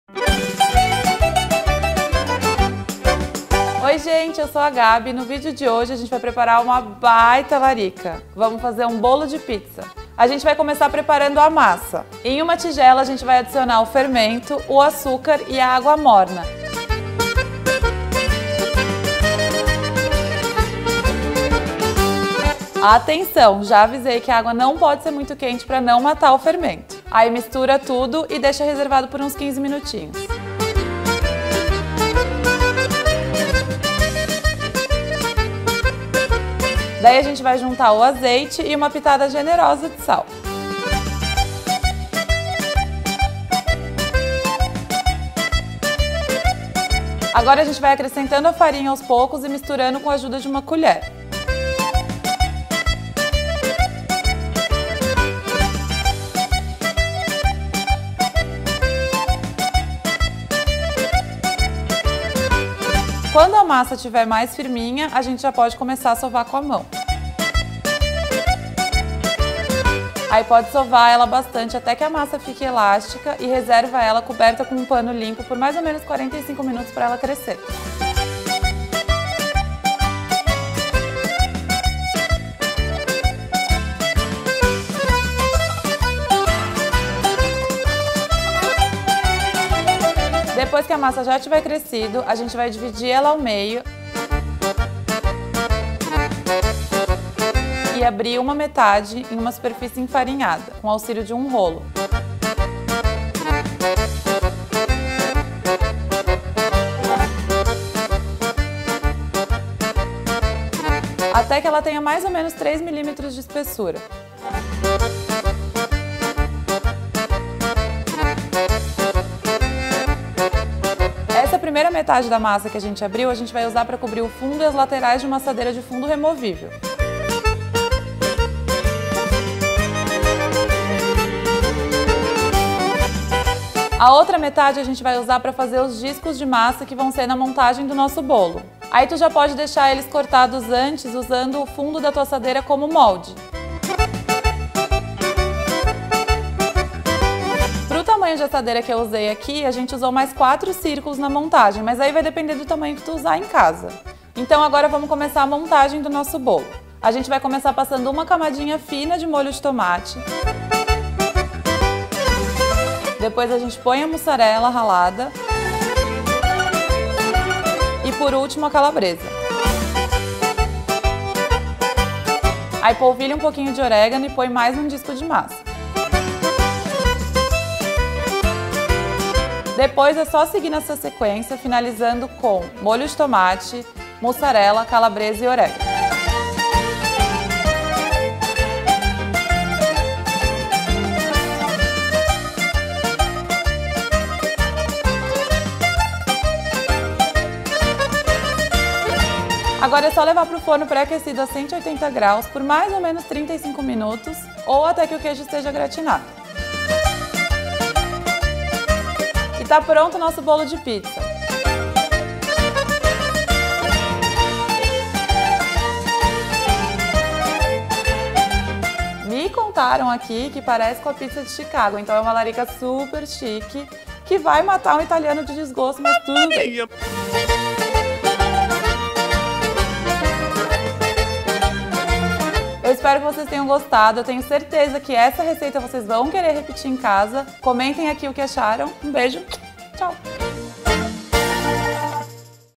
BOLO DE PIZZA É MELHOR AINDA | Receitas Comentadas
Pizza já é bom, mas que tal preparar um novo formato dessa receita tão deliciosa? Aprenda a preparar um incrível bolo de pizza com a Gabi. Todo mundo vai amar!